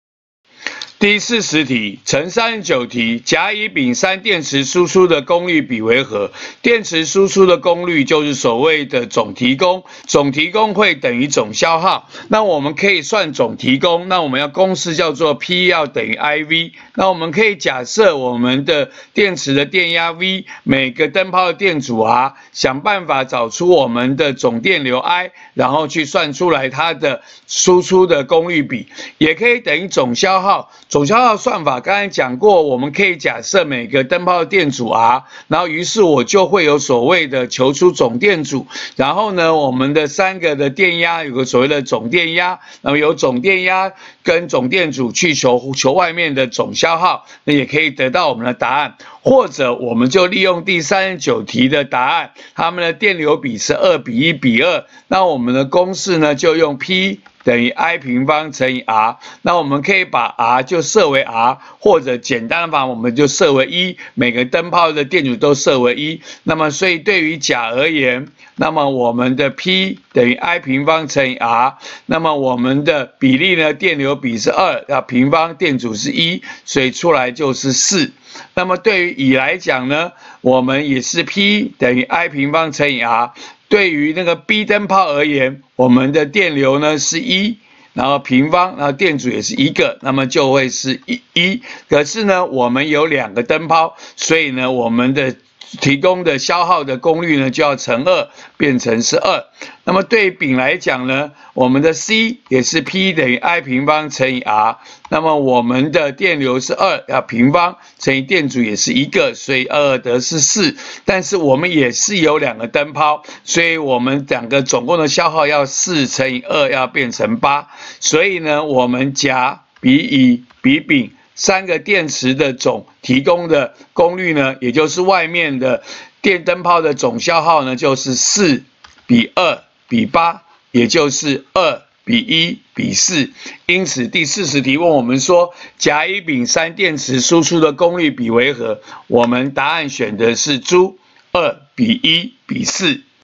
106學年下9年級理化一段第40題
承39題，問電池提供電功率比。